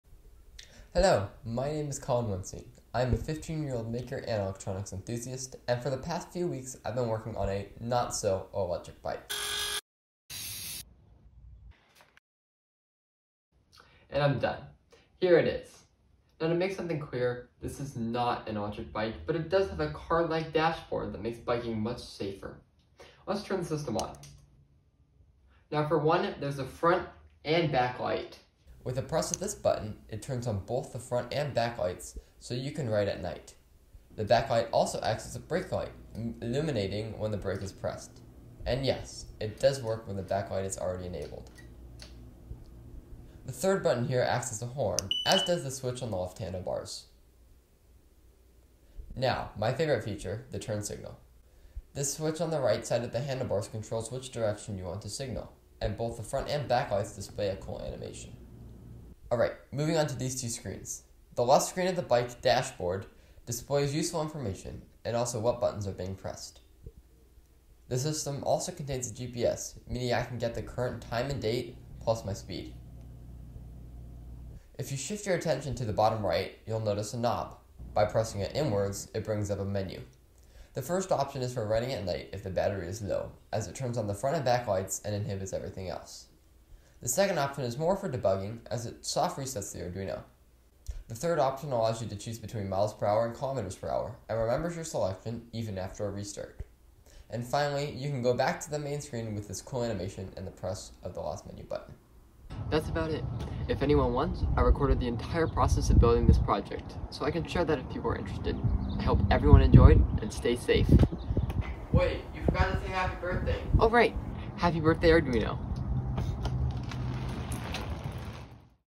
A (not so) Electric Bike
This is a submission for the Arduino Day 2021 Challenge.

Since I’ve been biking a lot lately, I wanted to add a system to help me better communicate with cars. I added a brake light, a front light, a turn signal, a horn, and a GPS speedometer to my bike. All of it is powered by an Arduino Pro MEGA and displays its info on two 1.3 inch screens.

Because I do cross country and soccer, I usually am able to get decent exercise each day. However, due to a recent injury, my only method of exercise is biking. While biking however, I wanted a way to communicate with cars, so I built this project.

I would like to improve it by adding GPS tracking so I can see where I went. It would also be interesting to experiment with a touchscreen instead of a rotary encoder to adjust settings.